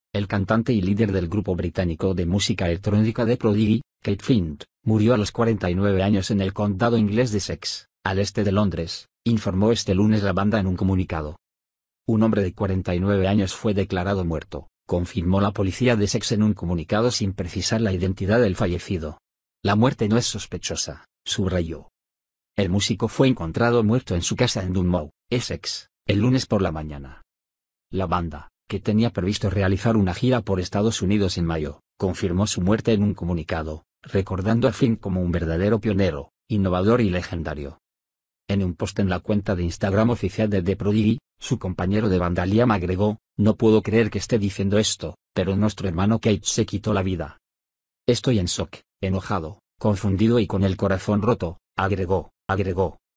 Encuentran muerto a Keith Flint fallece vocalista de The Prodigy
El cantante y líder del grupo británico de música electrónica The Prodigy, Keith Flint, murió a los 49 años en el condado inglés de Essex, al este de Londres, informó este lunes la banda en un comunicado.

“Un hombre de 49 años fue declarado muerto”, confirmó la policía de Essex en un comunicado sin precisar la identidad del fallecido. “La muerte no es sospechosa”, subrayó.

El músico fue encontrado muerto en su casa en Dunmow, Essex, el lunes por la mañana.

La banda, que tenía previsto realizar una gira por Estados Unidos en mayo, confirmó su muerte en un comunicado, recordando a Flint como un “verdadero pionero, innovador y legendario”.

En un post en la cuenta de Instagram oficial de The Prodigy, su compañero de banda Liam agregó: “No puedo creer que esté diciendo esto, pero nuestro hermano Keith se quitó la vida”.

“Estoy en shock, enojado, confundido y con el corazón roto”, agregó.